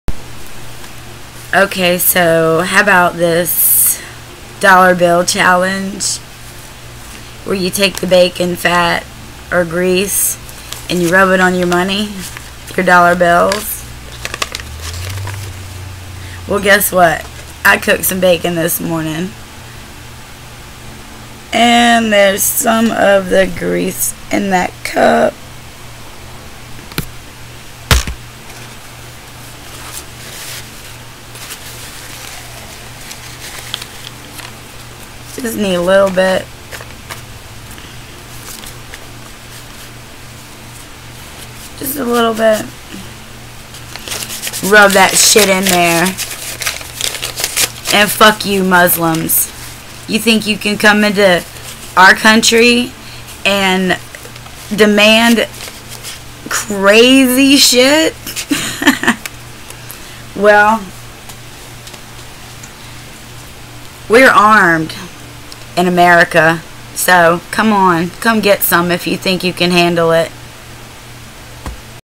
the bacon bill challenge
OK LETS DO THE "BACON BILL CHALLENGE"!!!
Take out all the bills in your wallet and smear a little BACON FAT on them.
you dont need much for this experiment .
Now just spend as you normally would and get those bills into circulation !!
If thousands of us do this and the word gets out that we are putting
BACON FAT on our money ...
how will these muslim business and cashiers react???
lets see what is stronger their faith or greed ...
the pig may turn out to be our hero !!!=) happy smearing